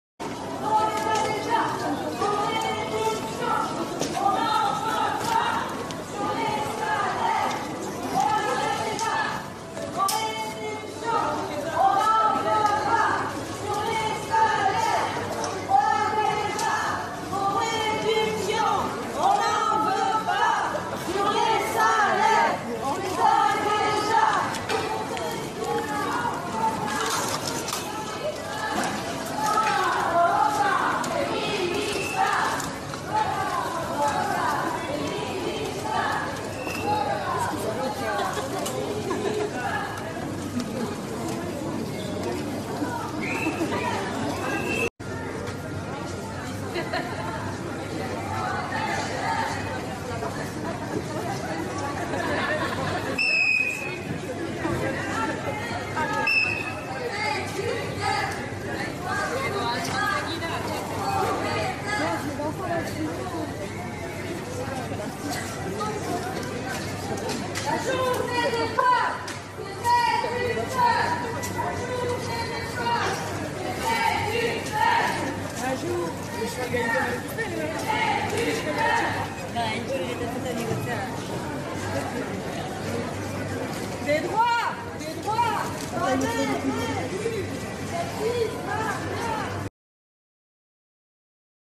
Greba feminista: Angeluko Leclerc saltegira sartu dira grebalariak
| Angeluko Leclerc saltegira sartu dira grebalariak, publizitate sexista salatzeko.